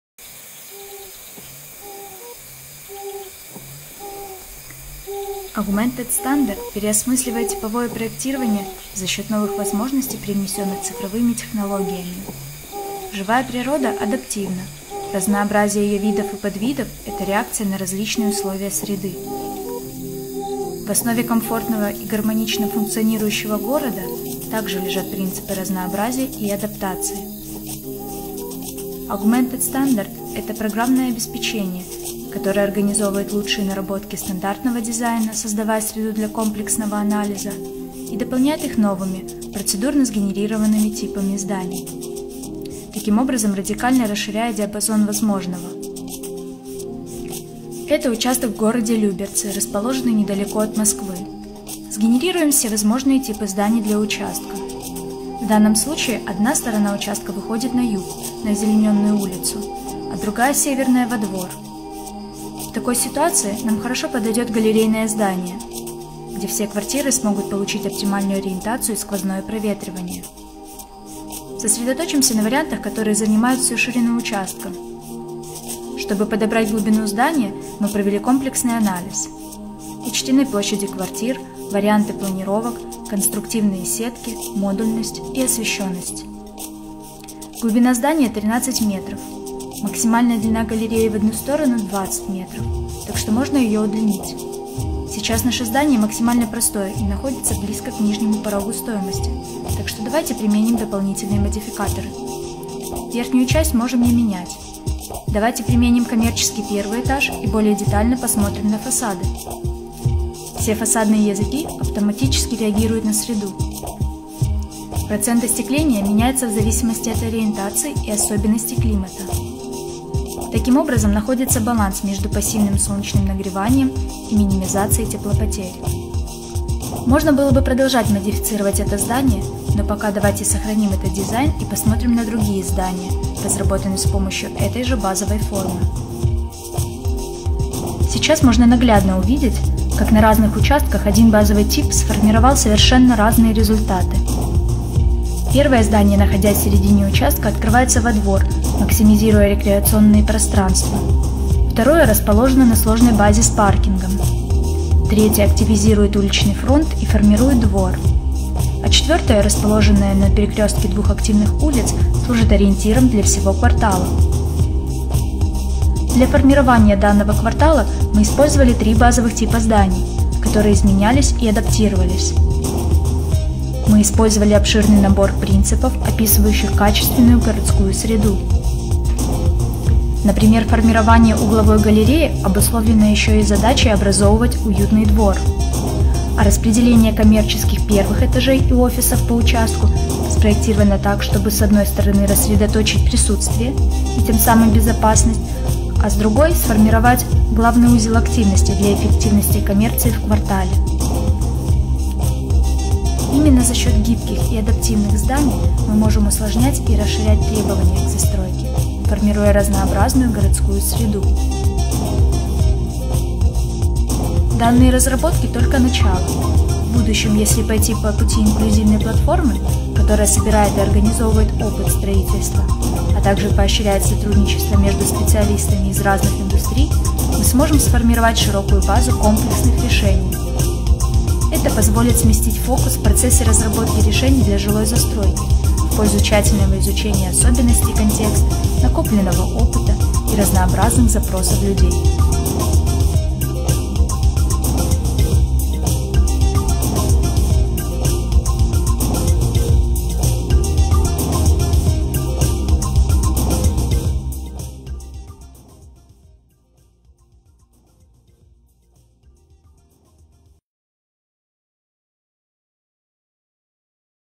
Augmented Standard Project
How can information technologies be leveraged for production of better cities?

Augmented Standard, a project led by architect and Strelka alumnus Maksym Rokmaniko, aims to develop a softwarized way of creating buildings that are essentially standard, but are more adapted to the environment and the needs of inhabitants. The broad idea behind Augmented Standard is to build a tool that will allow people to have their say.

Team: Maksym Rokmaniko, Yevheniia Berchul, Mykola Holovko, Nikita Bielokopytov + Alex Platonov (post-production) and Pekka Airaxin (music).

Как использовать информационные технологии для лучшего городского планирования?

Проект Augmented Standard архитектора и выпускника «Стрелки» Максима Рохманийко на примере дома в Люберцах исследует то, как можно улучшить строительство жилья — сделать его более комфортным и отвечающим запросам будущих жителей.